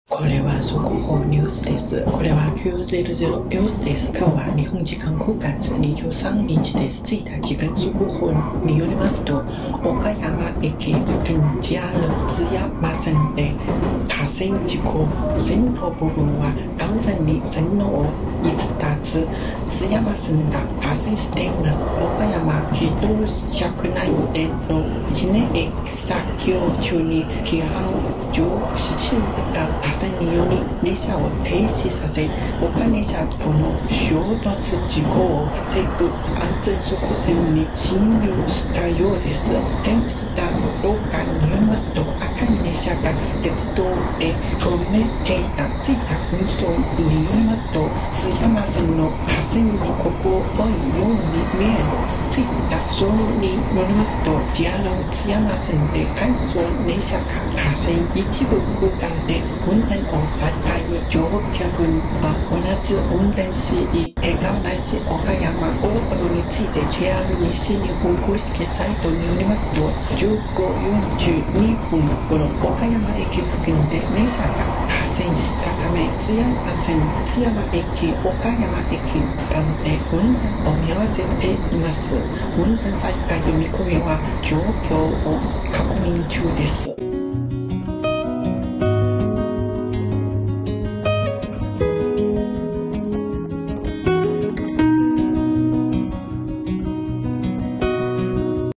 速報ニュース!ＪＲ津山線岡山駅付近で９月２３日１５時４２分頃列車が脱線した
速報ニュース!ＪＲ津山線岡山駅付近で
９月２３日１５時４２分頃列車が脱線した